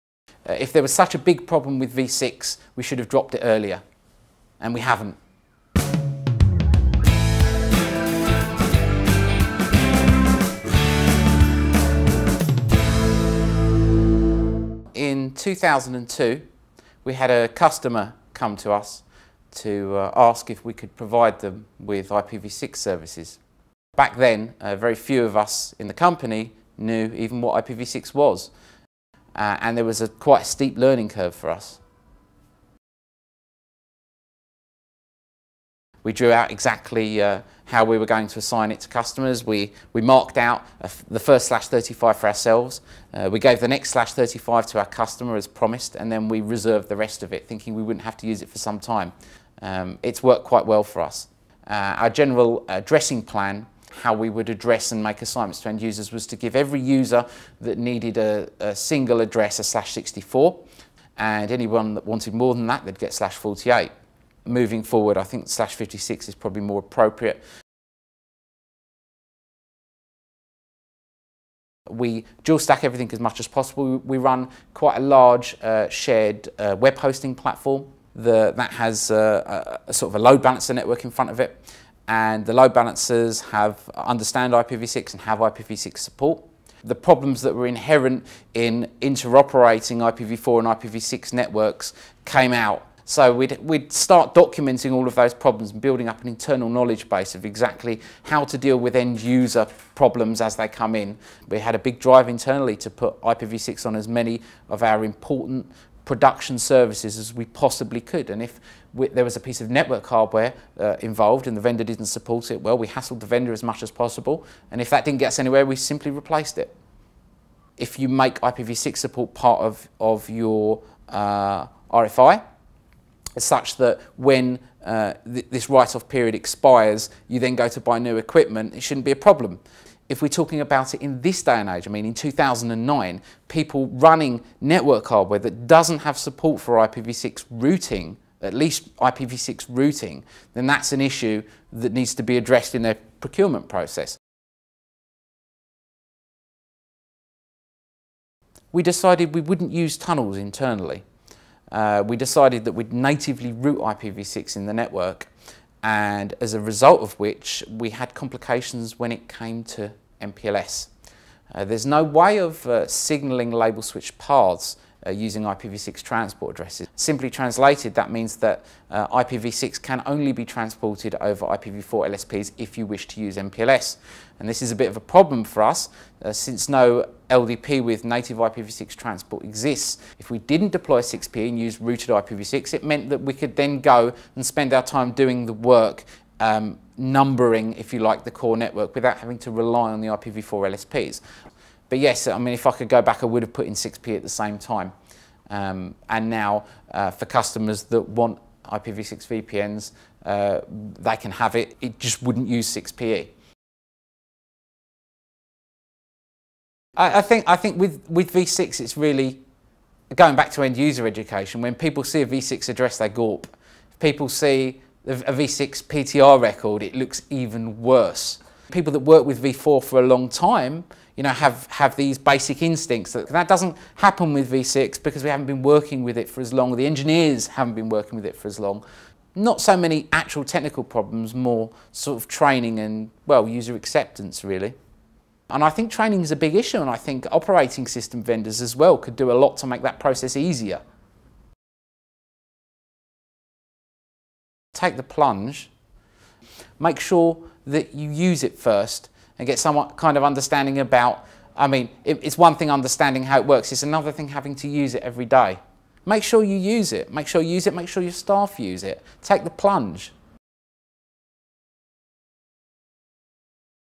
David Freedman of Claranet discusses their IPv6 deployment
David Freedman of Claranet discusses the implementation of IPv6 in their network. It covers the planning and deployment, including addressing plans and training, as well as the MPLS (Multiprotocol Label Switching) issues that they faced.